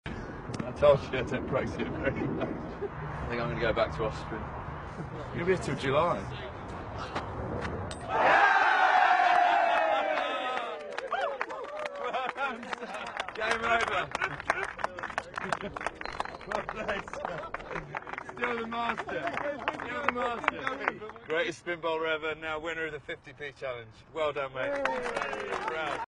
Best Bowler Of Cricket Wold Muttiah Muralitharan Cut the coin with Ball Edge
1. Best Bowler in International Cricket
2.Muralidaran; born 17 April 1972) is a former Sri Lankan cricketer who was rated the greatest Test match bowler ever by Wisden Cricketers' Almanack in 2002. He retired from Test cricket in 2010, registering his 800th and final wicket on 22 July 2010 from his final ball in his last Test match.[4] Muralitharan holds the world record for the most wickets in both test and one-day cricket.

3.Muralitharan's career was beset by controversy over his bowling action for much of his international career.